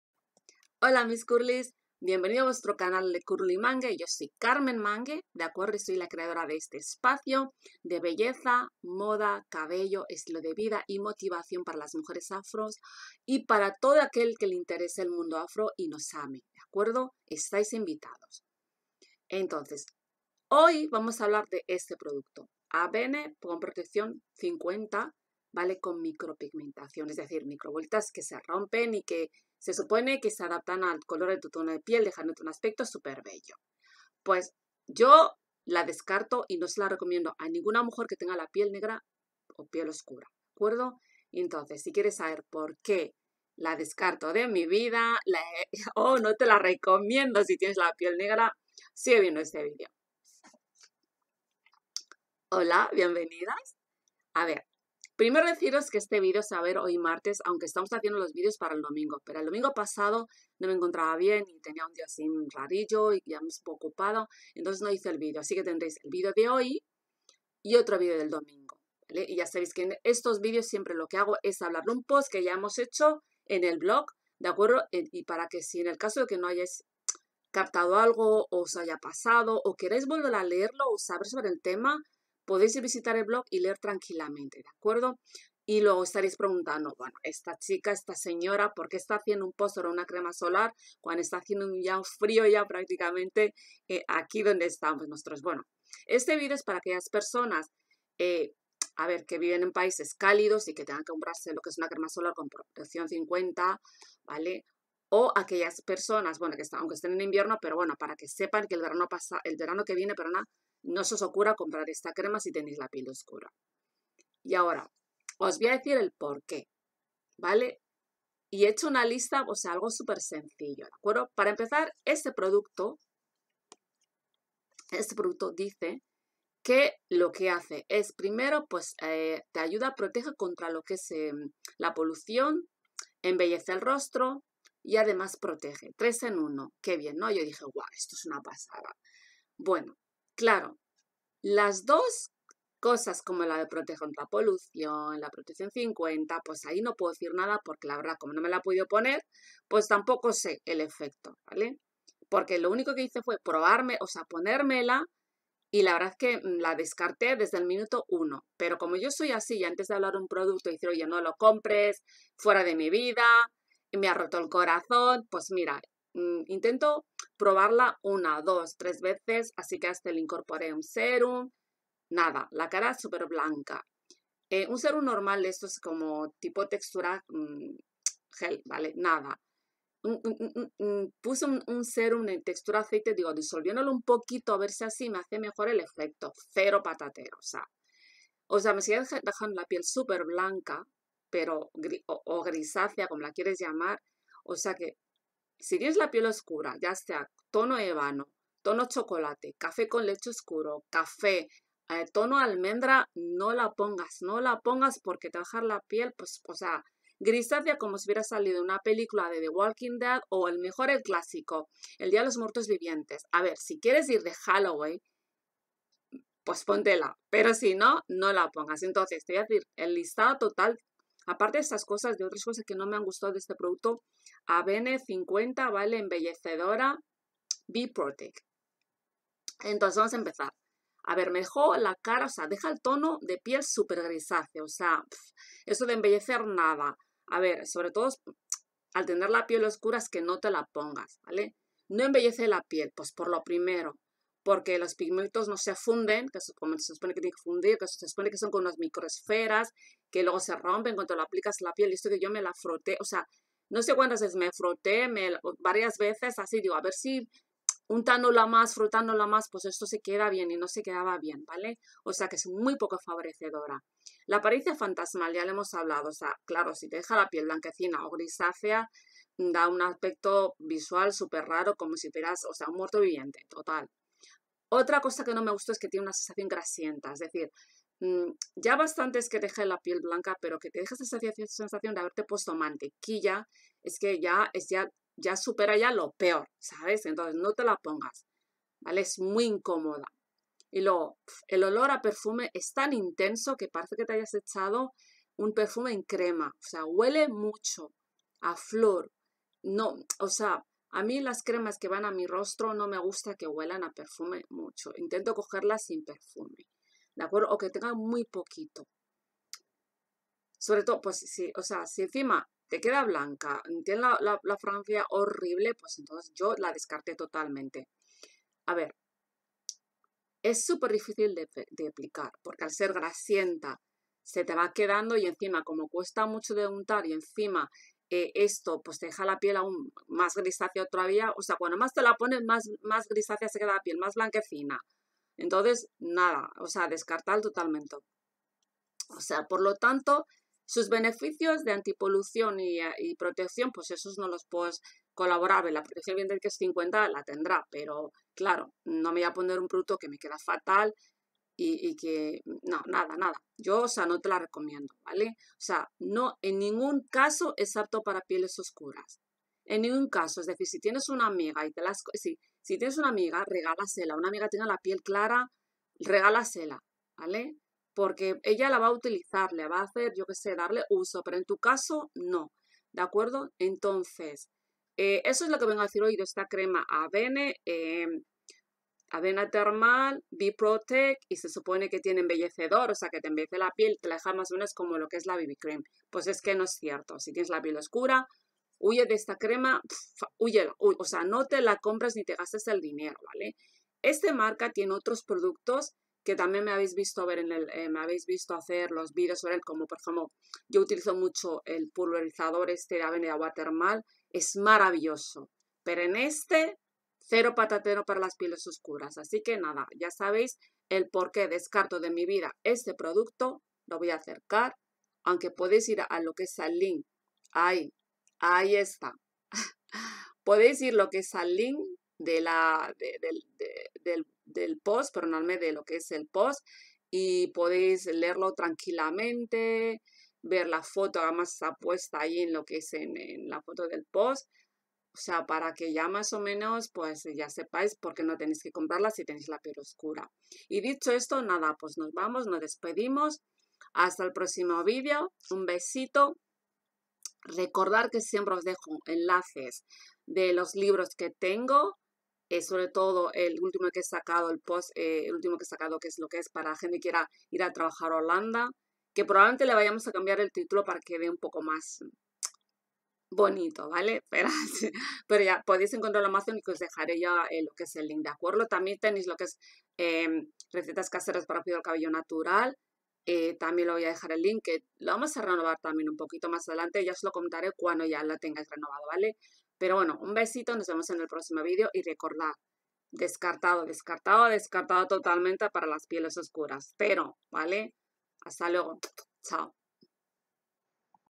7 Razones por las que el protector solar Avène B-PROTECT SPF 50+ NO es para pieles negras
Mi consejo es claro: no malgastes tu dinero en el protector solar Avène SPF 50 con micropigmentación, especialmente si tienes la piel oscura. Esto aplica a tonos de piel que van desde el ébano hasta el chocolate o el café con leche más intenso.

Este protector solar no está diseñado para nosotras. Aunque su fórmula con micropigmentos promete disolverse y unificar el tono de la piel, mejorando su aspecto de manera similar a una BB cream, la realidad es otra. En lugar de lograr un acabado uniforme y favorecedor, la crema deja una capa blanquecina que no se adapta a tonos de piel oscuros, generando un efecto opaco y poco atractivo.
Para  concer otras razones por las que he descartado este producto, visita el link del blog, que te dejo abajo.